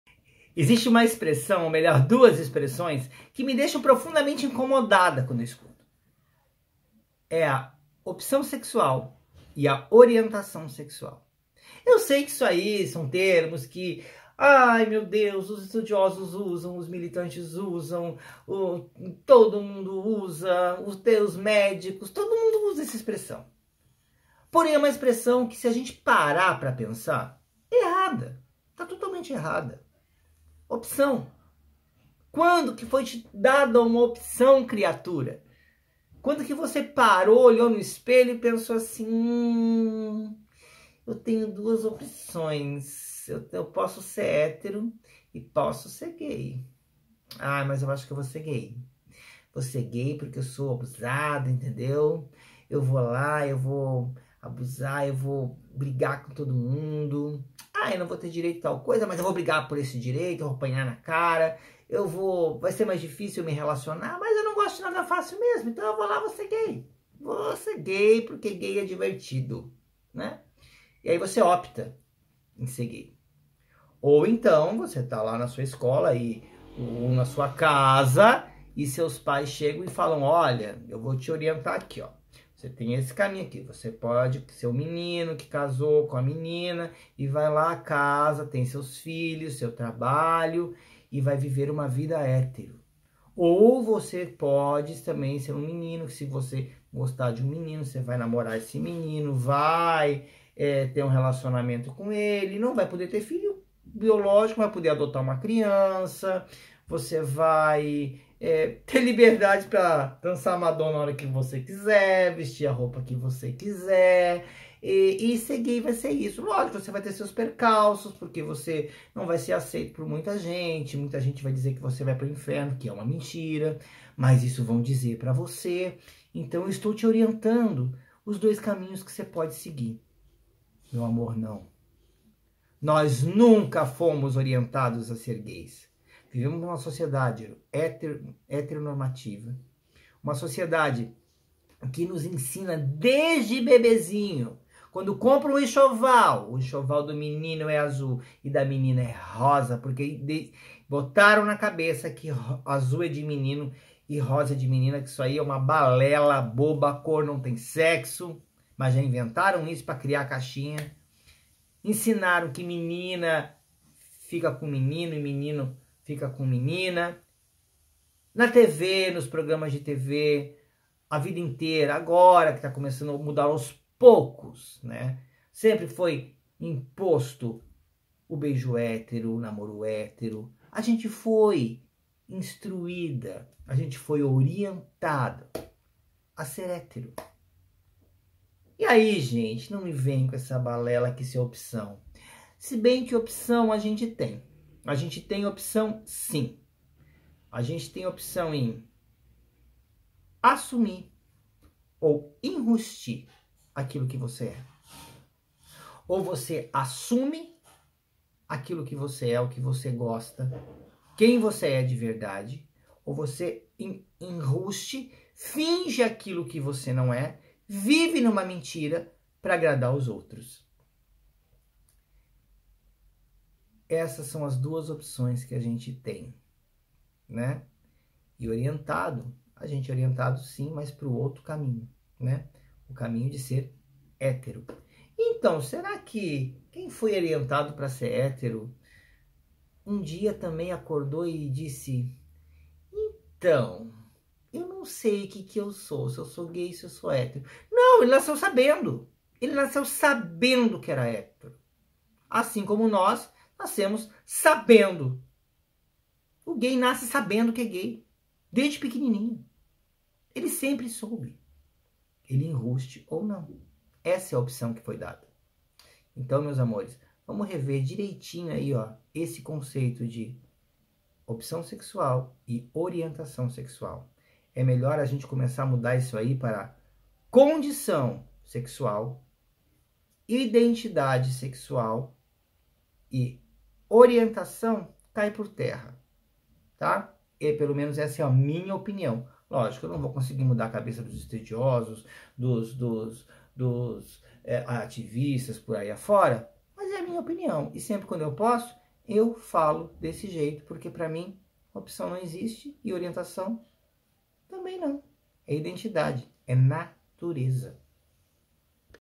Opção sexual ou orientação sexual???
Qual dessas duas expressões são as certas